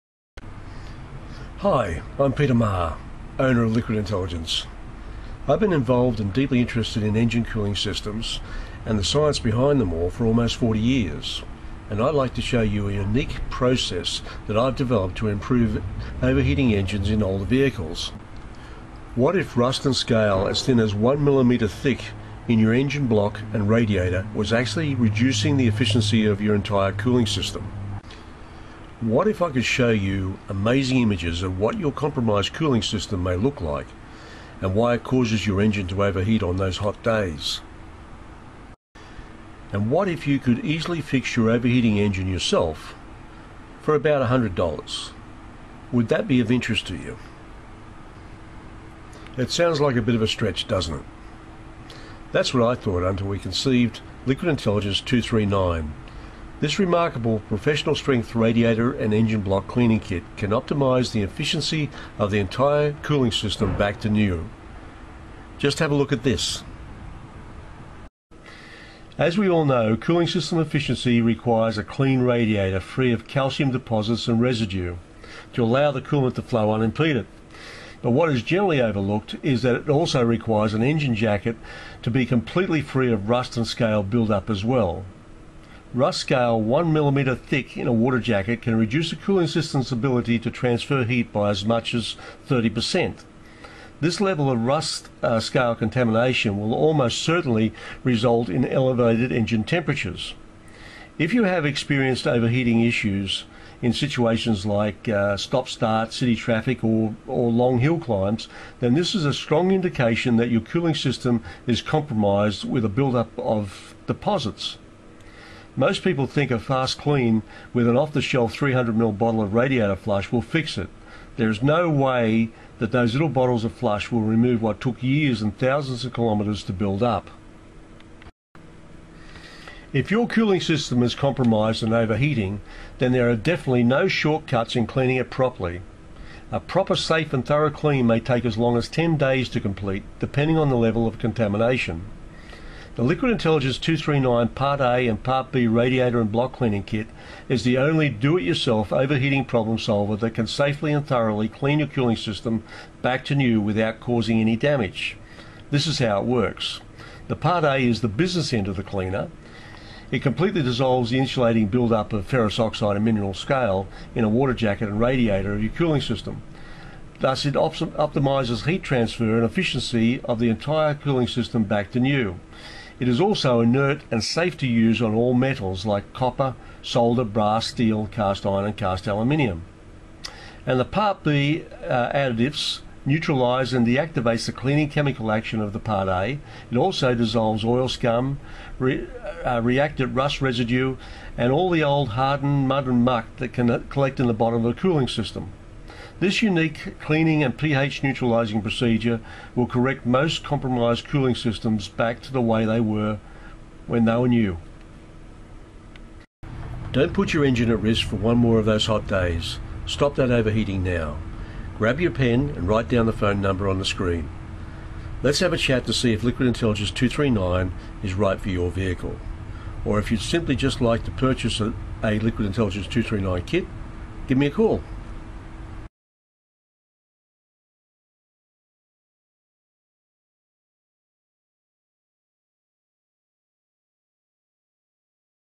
Engine Cooling System Cleaning Kit - Liquid Intelligence 239
Hi, I'm Peter Maher... owner of Liquid Intelligence... I have been involved and deeply interested in engine cooling systems. and the science behind it all... for almost forty years... and I would like to show you a unique process that I developed to improve overheating engines in older vehicles.

What if rust and scale as thin as one millimetre thick in your engine block and radiator was actually reducing the efficiency of your entire cooling system?

What if I could show you amazing images of what your compromised cooling system may look like and why it causes your engines to overheat on those hot days?

And what if you could easily fix your overheating engine yourself. for about $100.00. would that be of interest to you?

It sound like a bit of a stretch...doesn't it?

That's What I Thought until we conceived Intelligence 239. This Remarkable Professional Strength Radiator And Engine Block Cleaning Kit can optimize the efficiency of the entire cooling system back to new .Just have a look at this.